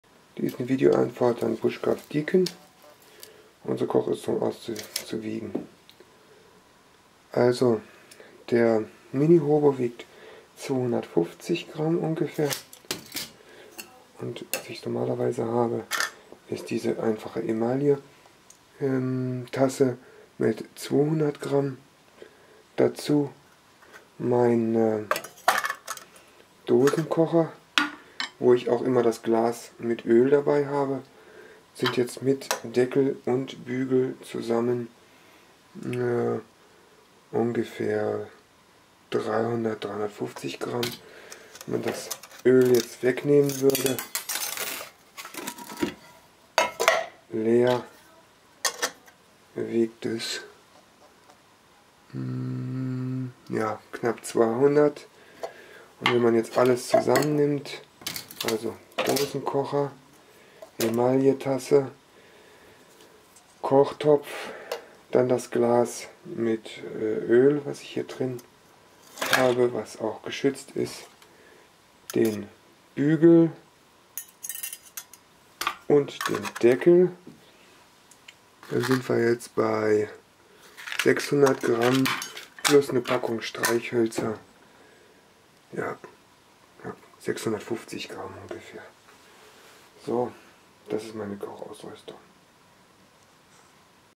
EDC Kochausrüstung wiegt ca. 650g
VA an BushcraftDicon's Challenge: Meine Kochausrüstung für eine Person besteht aus BushBox, Dosenkochtopf mit Bügel, Emailletasse, Glas mit Öl und einer Packung Streichhölzer.